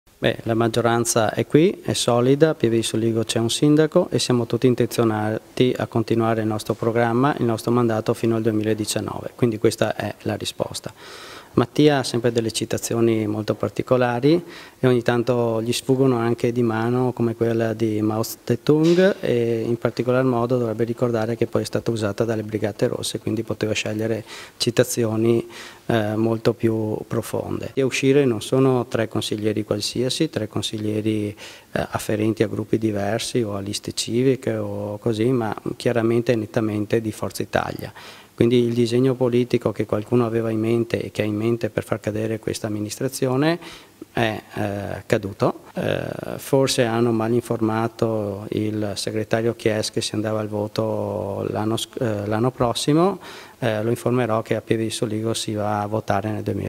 TREVISO TG - 15/02/2016 - IL SINDACO DI PIEVE: «MAGGIORANZA SOLIDA FINO AL 2019»
TREVISO TG (lunedì 15 febbraio 2016) - E la replica del sindaco non si è fatta attendere. La maggioranza è solida fino al 2019, dice Soldan